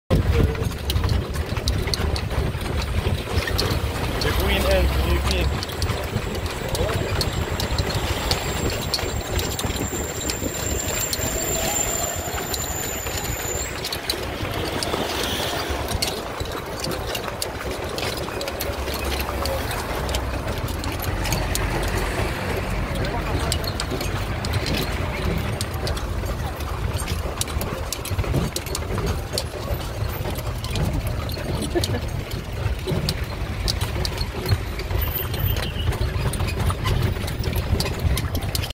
A Night in Hammemet City
My husband and I in a carousel going around the city. Carousels are parts of the entertaining in Hammemet.